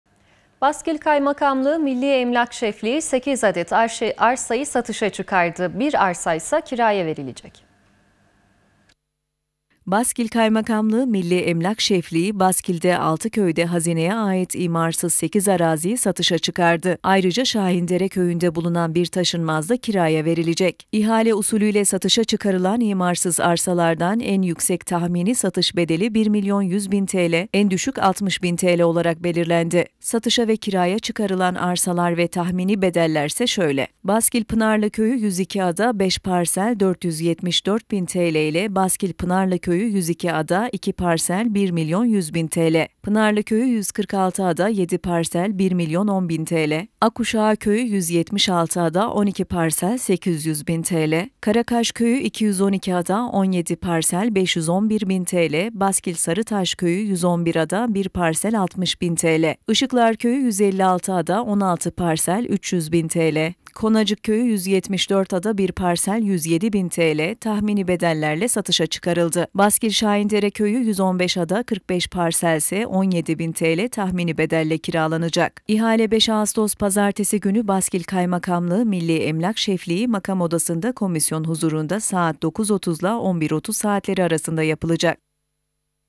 BASKİL’DE 8 ARSA SATIŞA ÇIKARILDI
BASKİL KAYMAKAMLIĞI MİLLİ EMLAK ŞEFLİĞİ 8 ADET ARSAYI
SATIŞA ÇIKARIRKEN 1 ADET ARSA İSE KİRAYA VERİLECEK